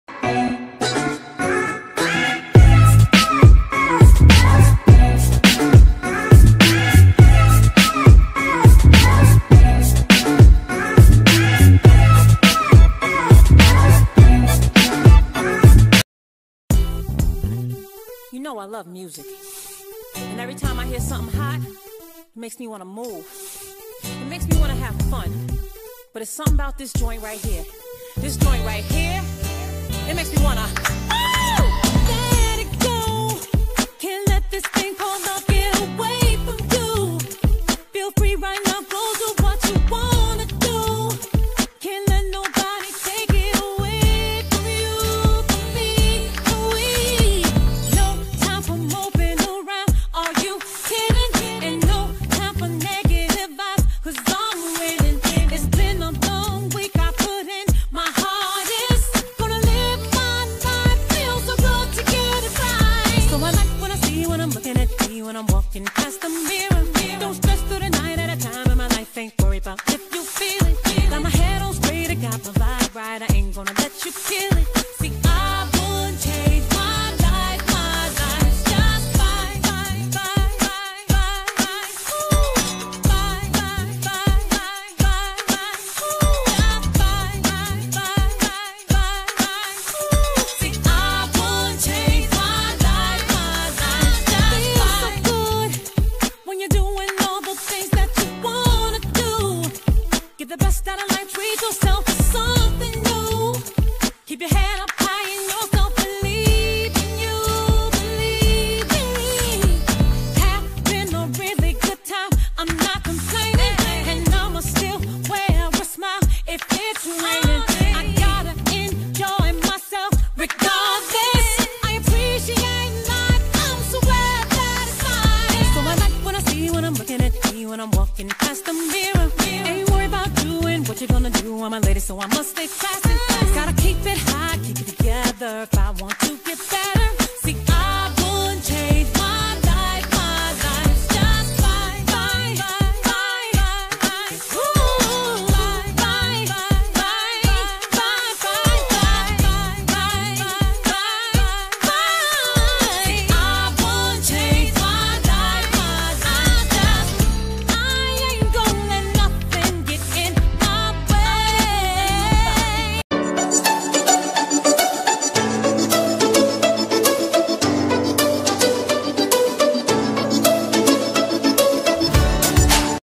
Makeup and Sisterlock Styles
These are some of my favorite makeup looks and Sisterlock Styles...